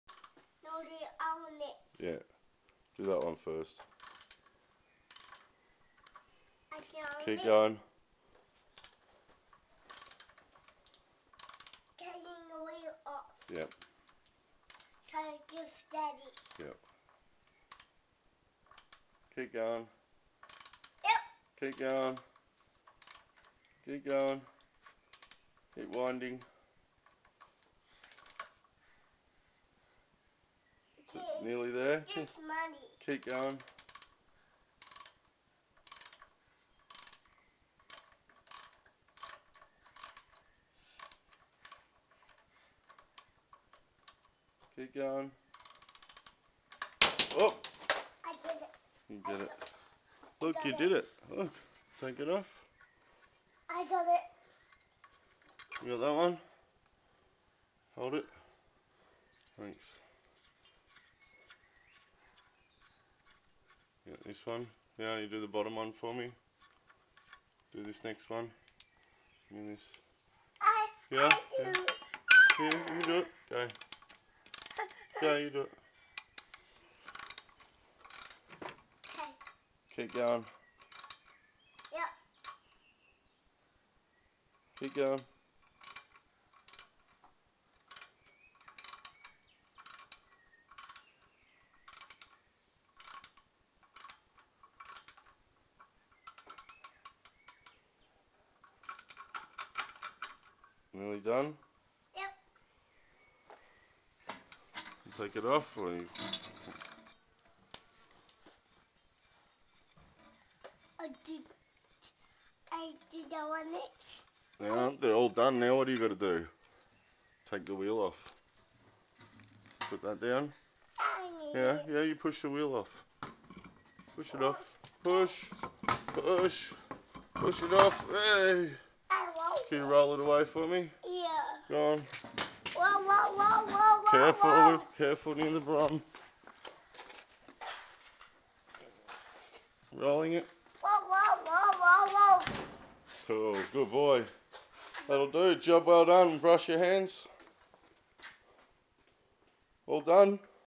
The Young Apprentice
My boy taking a wheel off my HG Brougham front end